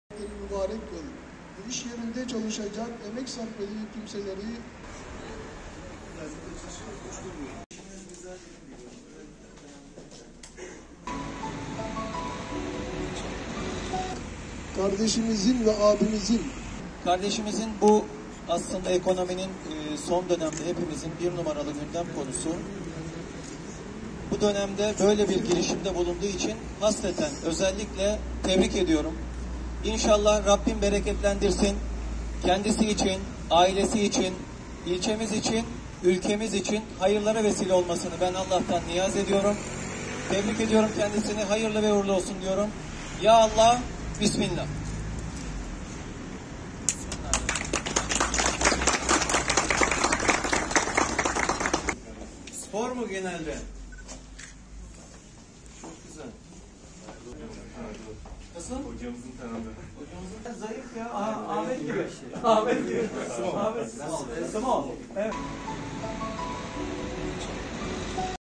"By Miraç" Erkek Giyim Mağazası Kestel'de Dualarla Açıldı
'By Miraç' isimli erkek giyim mağazasının açılışı Kestel'de gerçekleşti.
Kestel'de yeni bir işyeri daha hizmete girdi. ‘By Miraç’ adı ile hizmet verecek olan erkek giyim mağazasının açılış kurdelesini Kestel Belediye Başkanı Önder Tanır, Başkan Yardımcısı Hasan Aras, siyasi parti temsilcileri, dernek başkanları ve İşletme Sahibi Murat Çil birlikte kesti.
Ahmet Vefik Paşa mahallesi Lise Caddesi üzerinde hizmete giren mağazanın açılış programına belediye başkanı, kurum müdürleri, siyasi parti ve sivil toplum kuruluşları temsilcileri, çevre esnafı ve vatandaşlar katıldı.
Açılışta konuşma yapan Kestel Belediye Başkanı Önder Tanır, "Kardeşimizin, bu dönemde öyle bir girişimde bulunduğu için özellikle tebrik ediyorum. İnşallah Rabbim bereketlendirsin kendisi için, ailesi için, ilçemiz için ve ülkemiz için hayırlara vesile olmasını Allah'tan niyaz ediyorum. Hayırlı ve uğurlu olsun diyorum" dedi.
Açılış kurdelesinin kesilmesinin ardından iş yeri topluca gezildi. Açılış, tebriklerin kabulünün ardından verilen ikramlar ve yapılan sıcak sohbetlerle devam etti. İşletme sahibi Murat Çil, açılışa gelen bütün konuklarına teşriflerinden dolayı teşekkürlerini iletti.